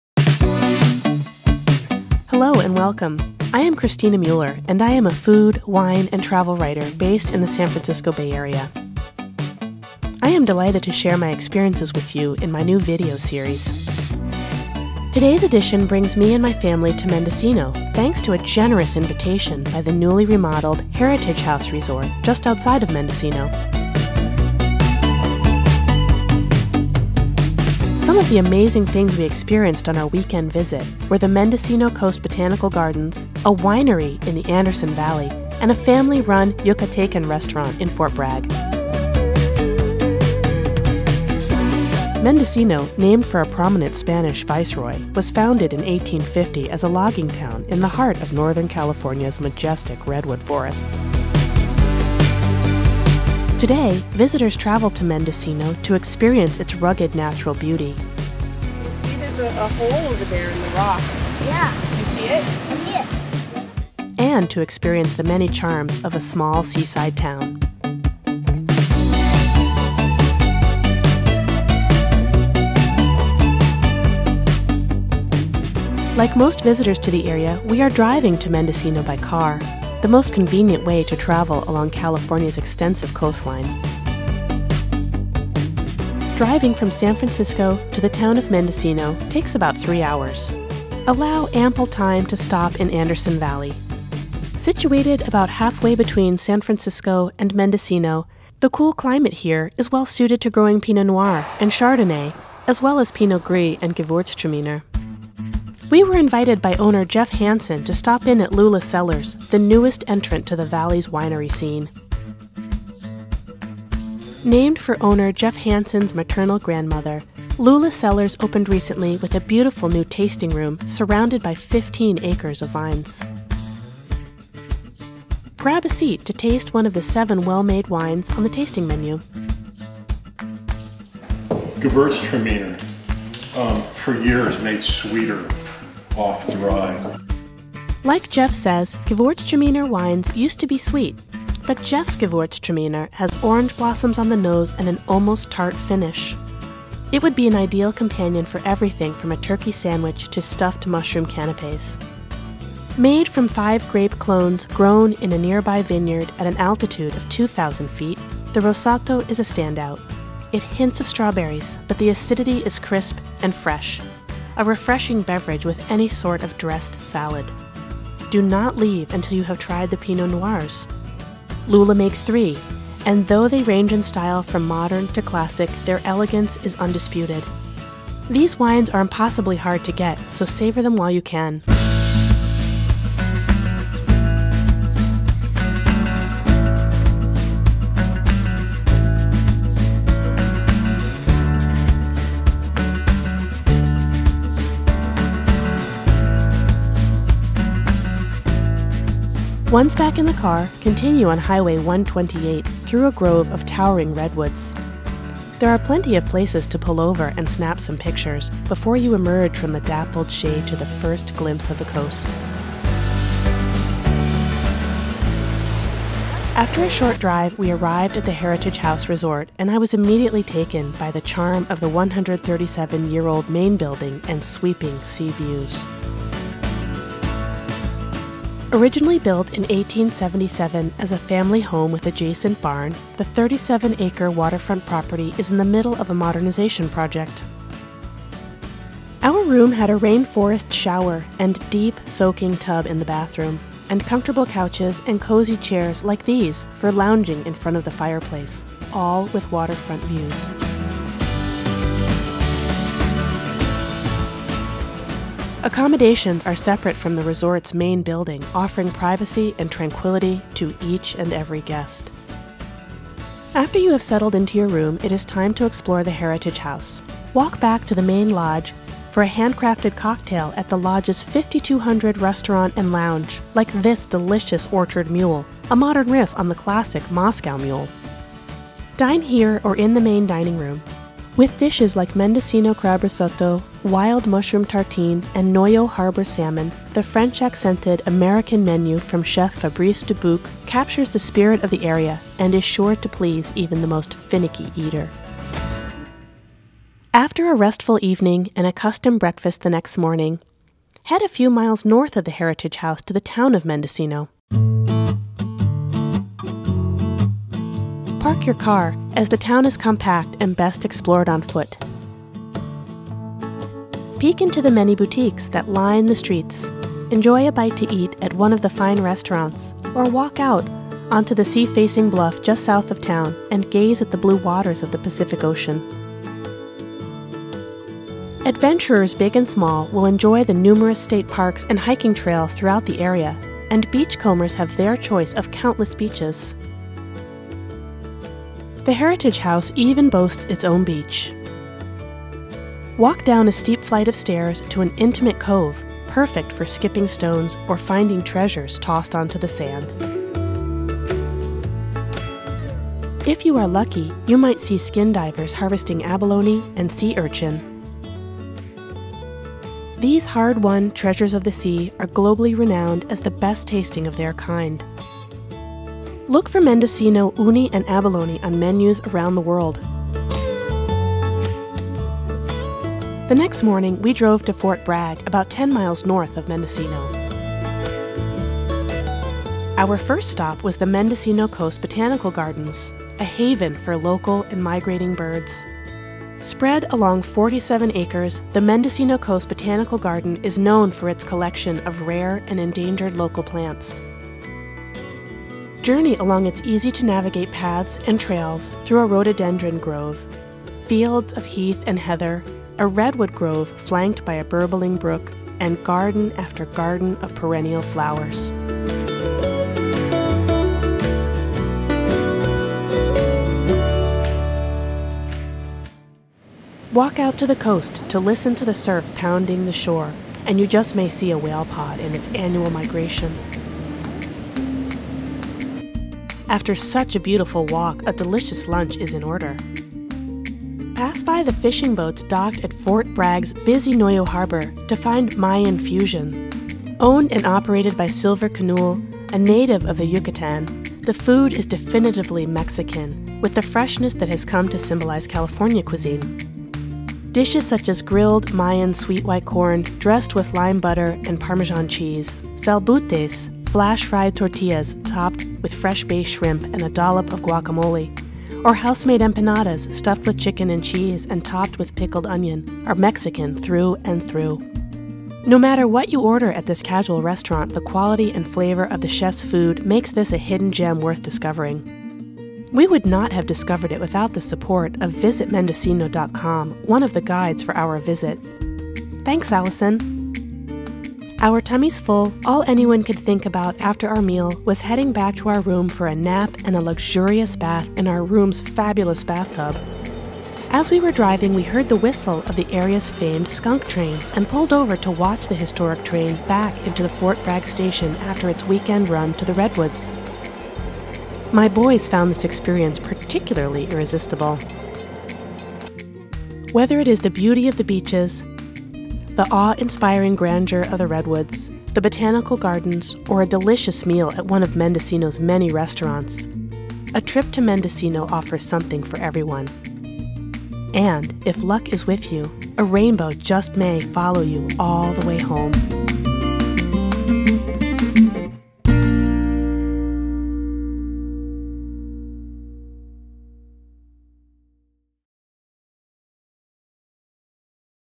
What to do when you're in Mendocino - A food, wine, travel and culture video blog
Join me and my family as we experience a weekend in Mendocino. Includes the restaurants, hotels and sights that are not to be missed in this rugged Northern California seaside town. To read more about the restaurants we visited, please read my post